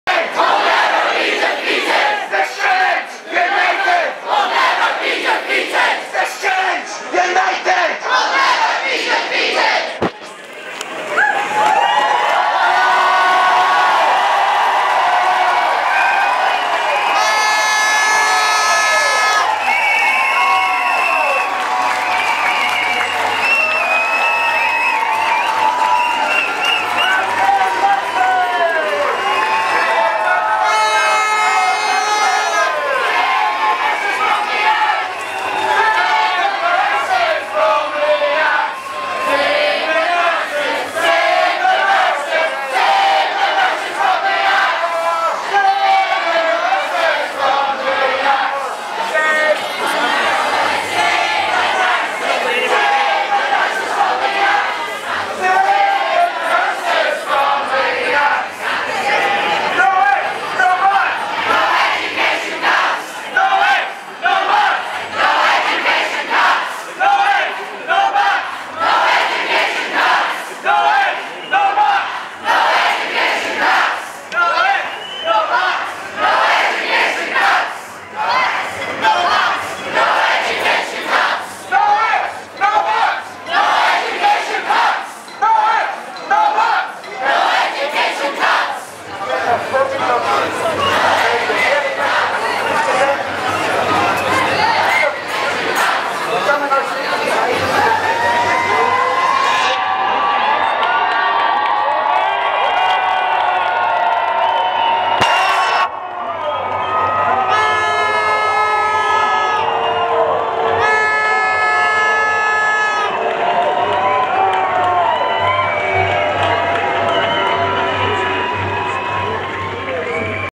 Students try to push forward to Glasgow Uni' Principals office
After the march though campus over a thousand students and staff walked through the University of Glasgow to outside Principal Anton Muscatelli office .